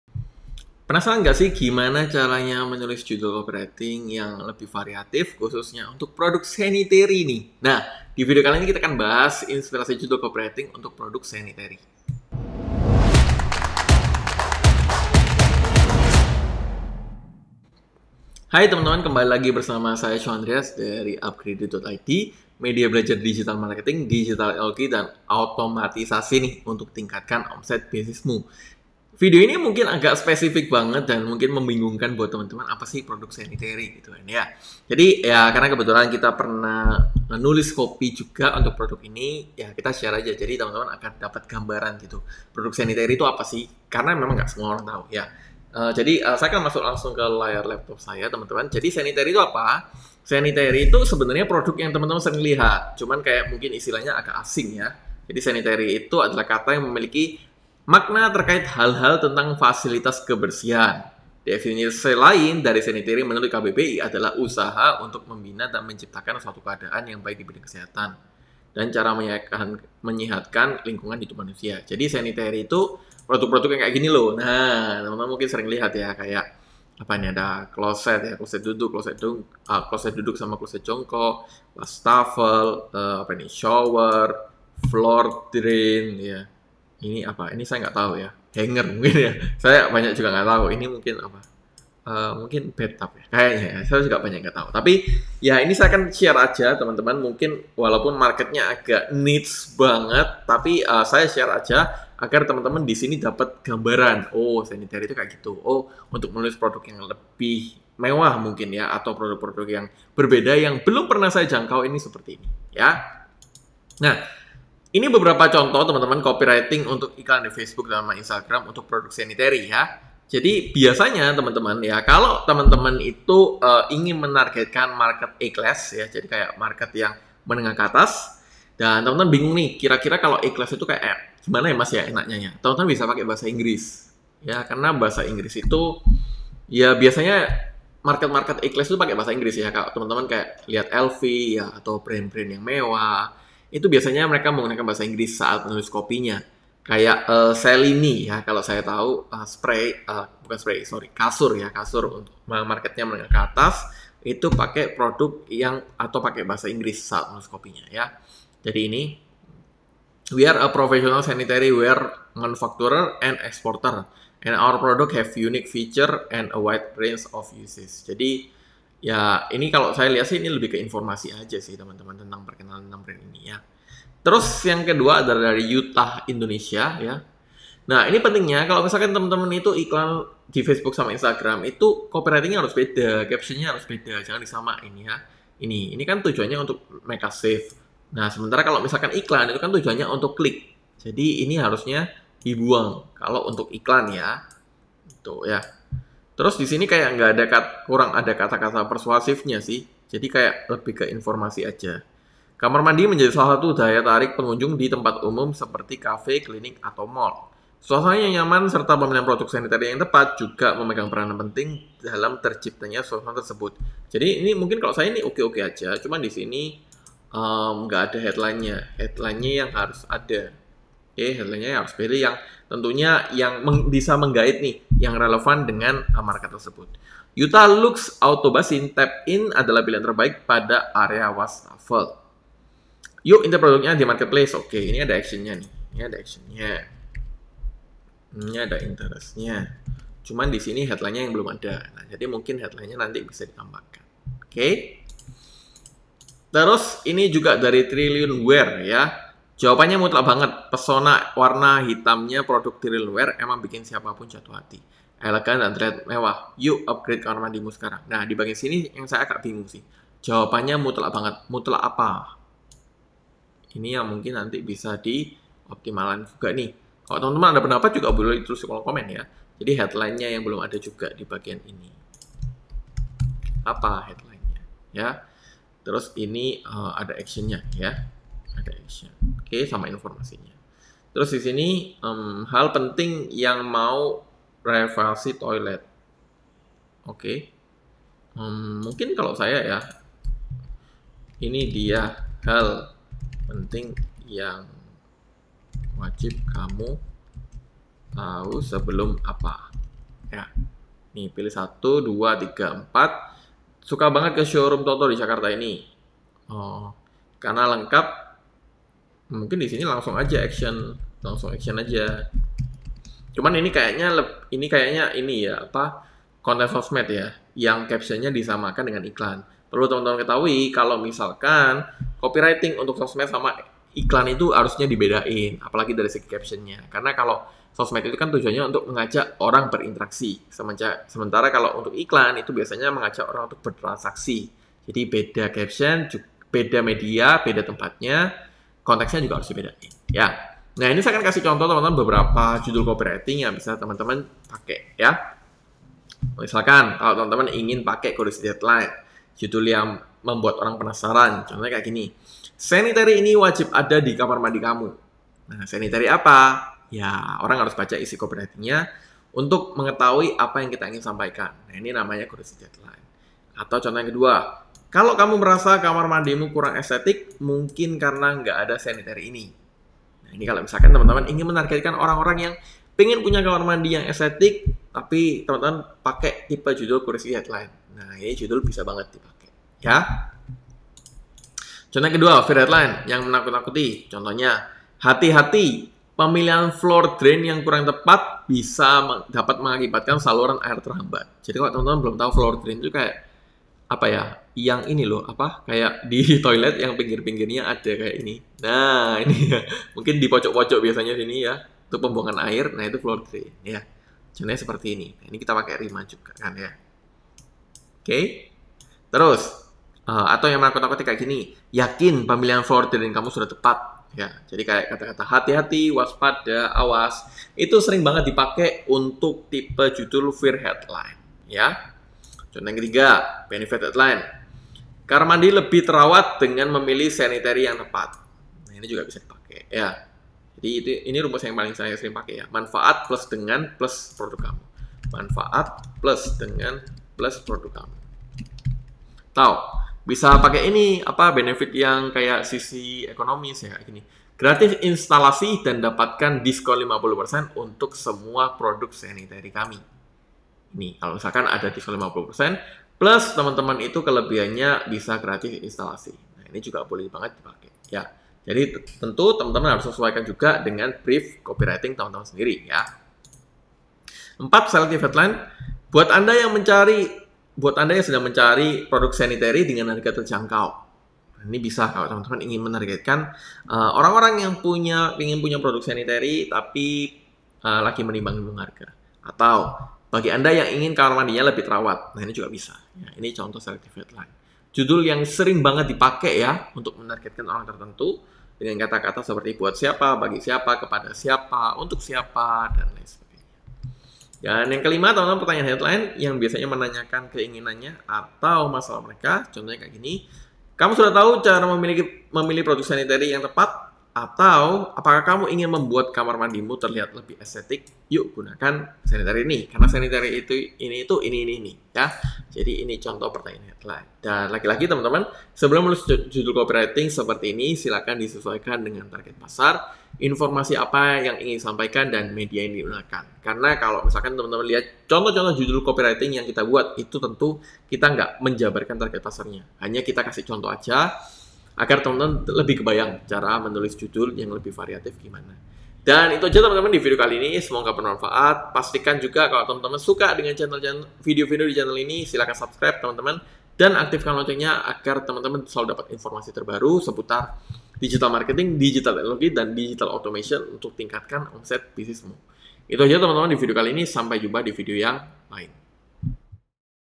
Inspirasi Judul COPYWRITING untuk Produk SANITARY
Produk sanitary yang kamu jual kurang dapat perhatian pembeli? Mungkin hook copywriting kamu kurang menarik, nih! Yuk belajar membuat judul copywriting yang unik dan produk sanitary kamu akan terlihat lebih menarik!